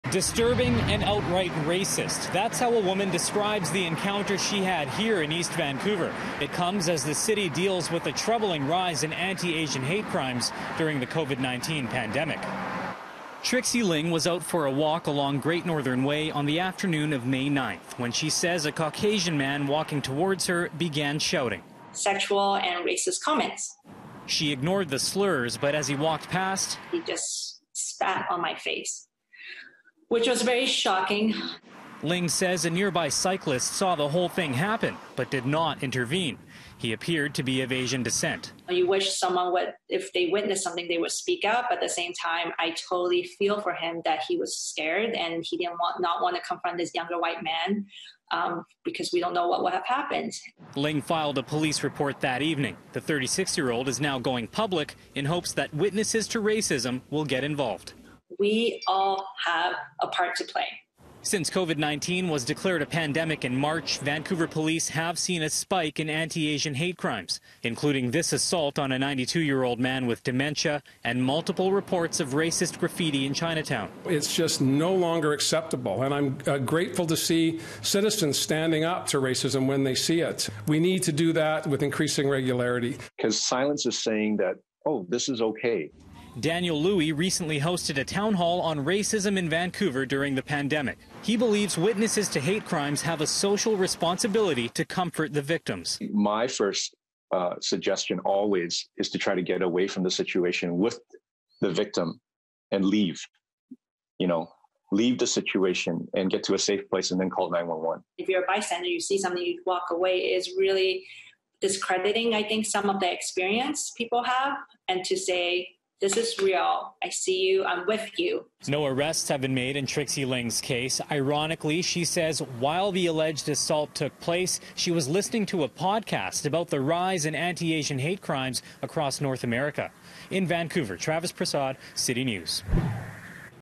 Woman spat on in alleged racist attack
A Vancouver woman is sharing her experience of an unprovoked, racist assault in east Vancouver. As Travis Prasad reports, she wants witnesses to hate crimes to start stepping in.